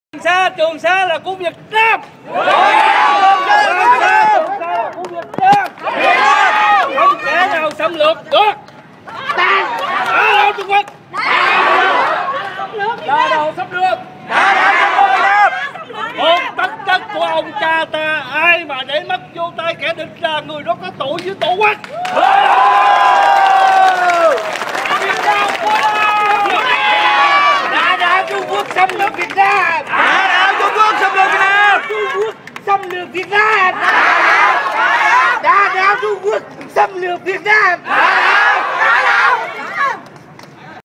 Mít - tinh phan doi Trung Quoc xam luoc o sai gon 09/12/2012
Đất đai của ông cha ta, ai mà làm mất là có tội với tổ quốc.